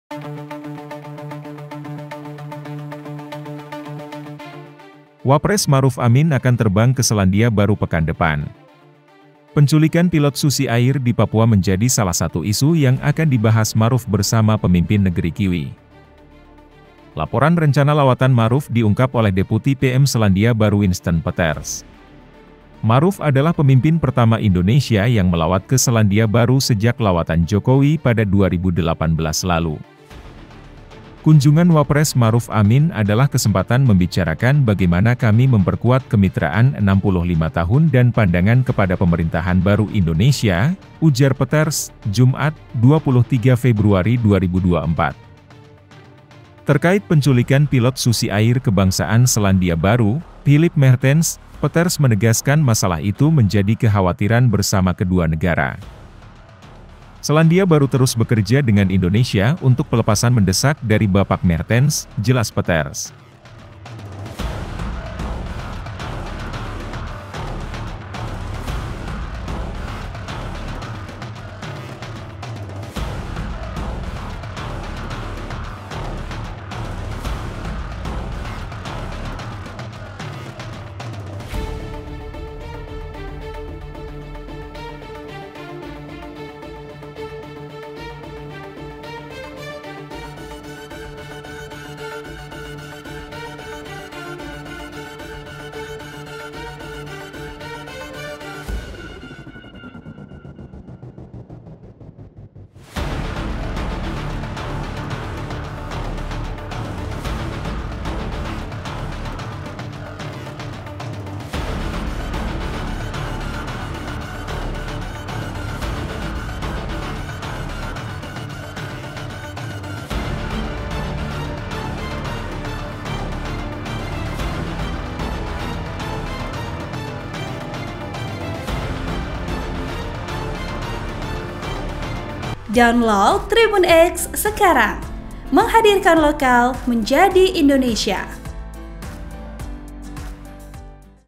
Ma'ruf Amin Akan ke Selandia Baru, Bahas Pembebasan Pilot Susi Air
POS-KUPANG.COM - Wapres Ma'ruf Amin akan terbang ke Selandia Baru pekan depan. Penculikan pilot Susi Air di Papua menjadi salah satu isu yang akan dibahas Ma'ruf bersama pemimpin Negeri Kiwi.

Laporan rencana lawatan Ma'ruf diungkap oleh Deputi PM Selandia Baru Winston Peters. Ma'ruf adalah pemimpin pertama Indonesia yang melawat ke Selandia Baru sejak lawatan Jokowi pada 2018 lalu.

"Kunjungan Wapres [Ma'ruf Amin] adalah kesempatan membicarakan bagaimana kami memperkuat kemitraan 65 tahun dan pandangan kepada pemerintahan baru Indonesia," ujar Peters, Jumat (23/2/2024).

Terkait penculikan pilot Susi Air kebangsaan Selandia Baru, Philip Mehrtens, Peters menegaskan masalah itu menjadi kekhawatiran bersama kedua negara.

"Selandia Baru terus bekerja dengan Indonesia untuk pelepasan mendesak dari Bapak Mehrtens," jelas Peters.

Vp: Dhev